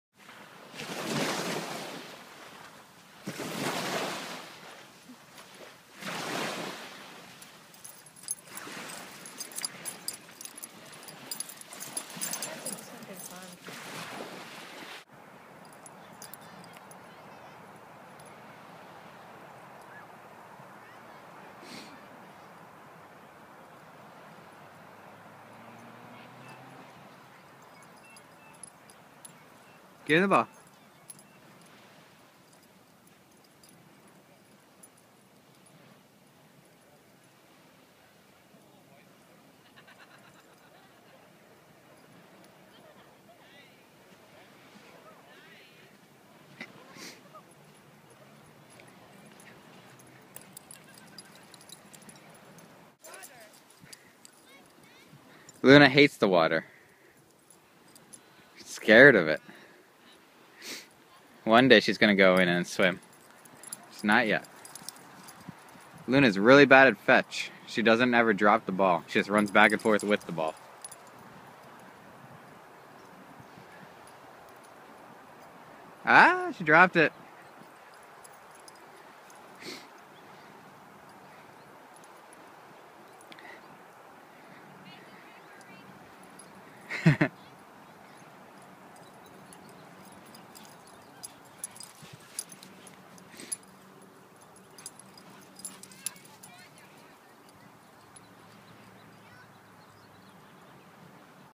Luna at English Bay Beach in Vancouver, BC - Siberian Husky and Wolf mixed puppy at 5 months
Luna attempting to play fetch at the beach